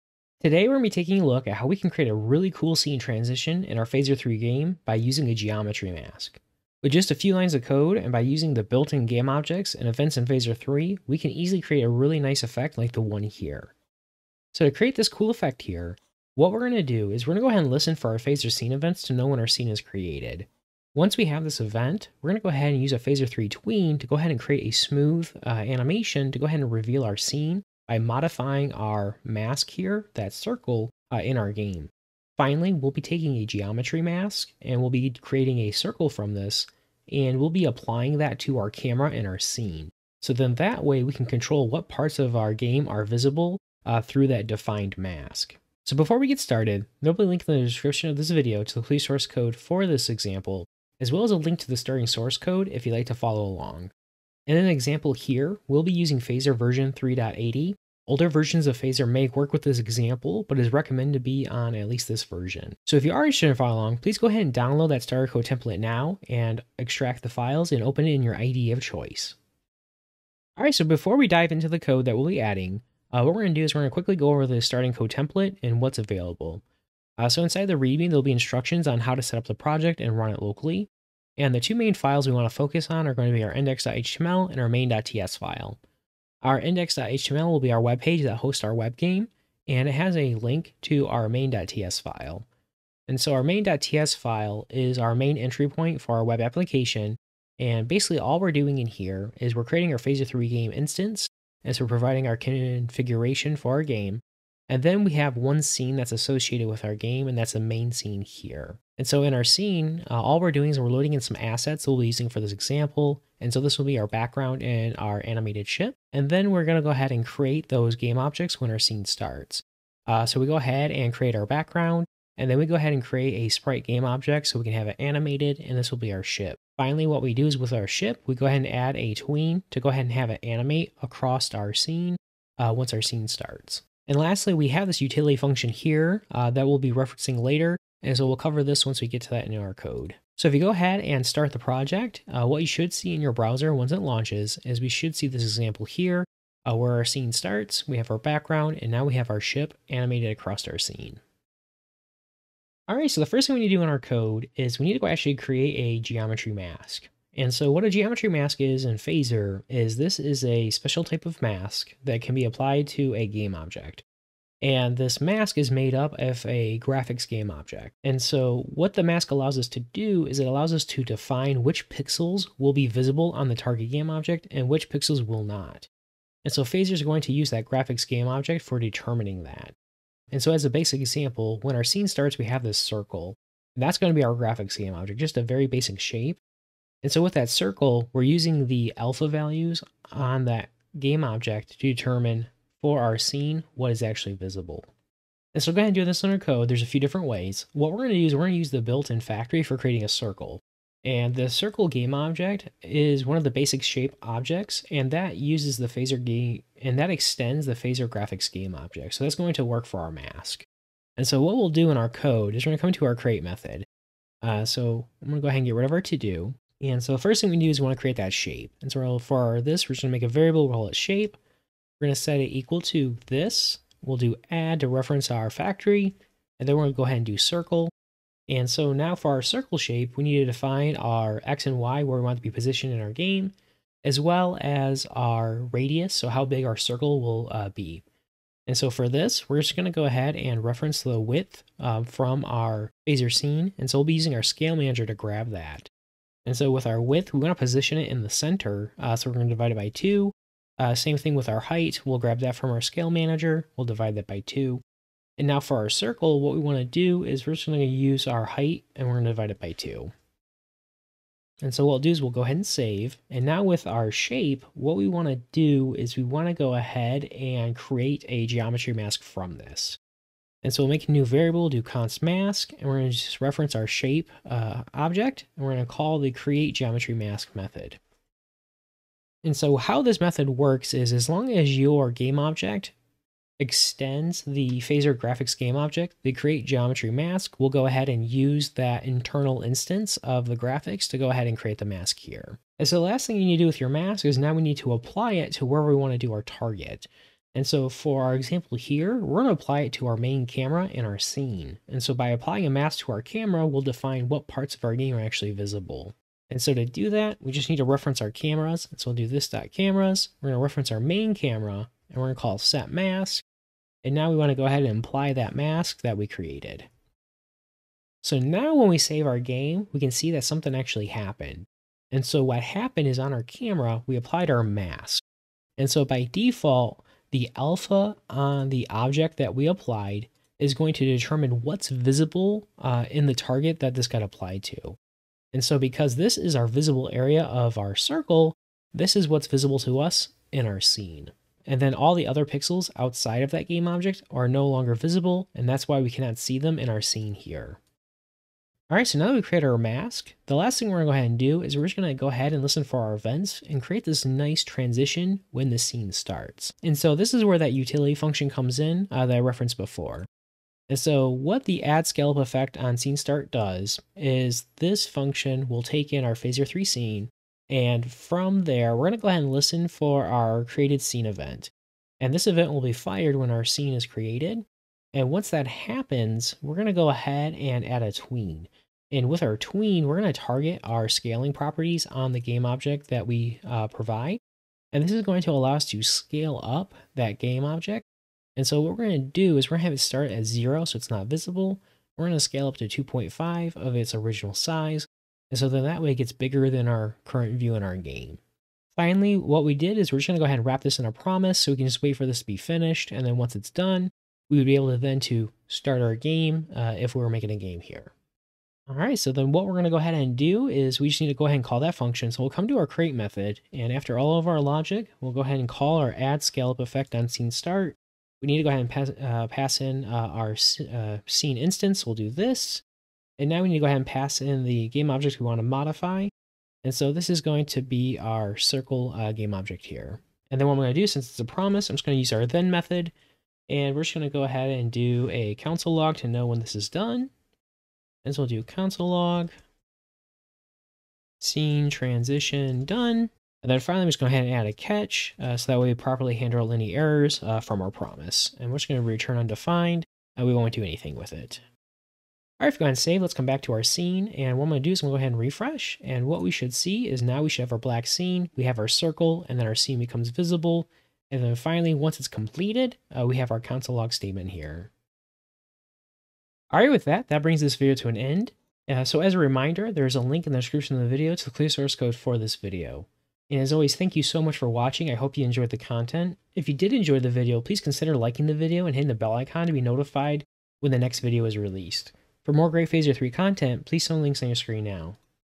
Phaser 3 Tutorial: Custom Level Transitions With Geometry Masks!
In this quick tutorial, we take a look at how we can create really nice custom level transitions by using the built in Phaser 3 Geometry Mask objects. With the geometry masks, we can use various game objects in our Phaser 3 scene to mask out parts of our game by applying the geometry mask to our in game camera. By combining some of the built in game objects in Phaser, we can create a really nice scene transition.

Video Chapters
0:00 - Intro
1:10 - Review starting code
2:32 - Review geometry mask
3:15 - Start of code
5:48 - Create transition
7:46 - Thanks and links to other content

The assets used in this demo were created You can find links to the content here